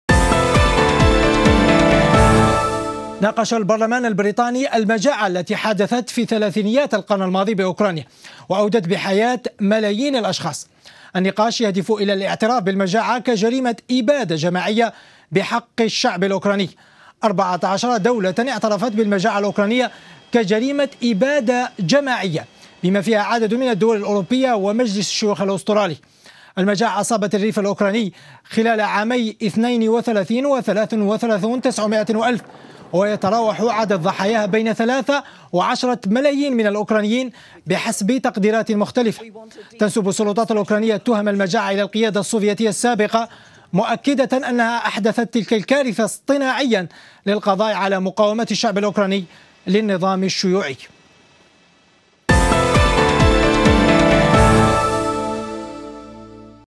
البرلمان البريطاني يناقش الاعتراف بالمجاعة الأوكرانية كجريمة إبادة جماعية
ناقش البرلمان البريطاني المجاعة التي حدثت في ثلاثينيات القرن الماضي بأوكرانيا، وأودت بحياة ملايين الأشخاص.

النقاش يهدف إلى الاعتراف بالمجاعة كجريمة إبادة جماعية بحق الشعب الأوكراني.

24 دولة اعترفت بالمجاعة الأوكرانية كجريمة إبادة جماعية، بما فيها عدد من الدول الأوروبية ومجلس الشيوخ  الأسترالي.